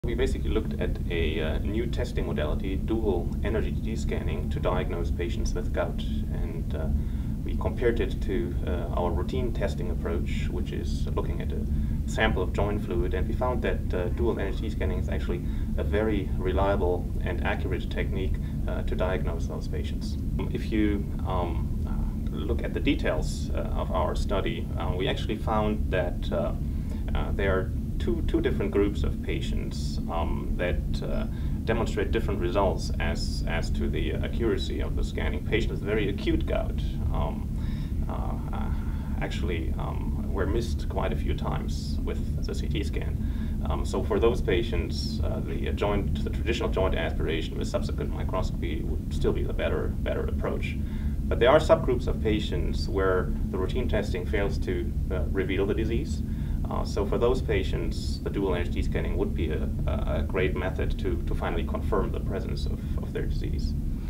CT scans shown to help in gout detection Mayo Clinic
Gout —the buildup of uric acid crystals in and around joints _ can be intensely painful and even disabling.  It is important to c atch the condition early.  A Mayo Clinic study has found that X-ray images known as CT  scans can help confirm gout in patients who are suspected of having the painful  condition but receive negative results from traditional tests. The type of CT scan researched, dual-energy computed tomography, is also valuable for diagnosing people who cannot be tested with the typical method of drawing fluid from joints, researchers found. The study is being presented at the American College of Rheumatology annual scientific meeting in Chicago.

Gout has historically been portrayed as a disease of kings,
 but in modern times, our diets have caught up with those of royalty, and gout now afflicts people from all walks of life. Men are likelier to develop gout, but women's risk rises after menopause, when their uric acid levels approach those of men. Treatment usually involves medication and dietary changes.

Physicians traditionally check for gout by using a needle to  draw fluid from affected joints and examining the fluid for uric acid crystals. Dual-energy CT scans  were recently modified to detect the crystals, and the study found the scans "very accurate'' in identifying patients with gout, says lead researcher Tim Bongartz, M.D., a rheumatologist at Mayo  Clinic in Rochester, Minn.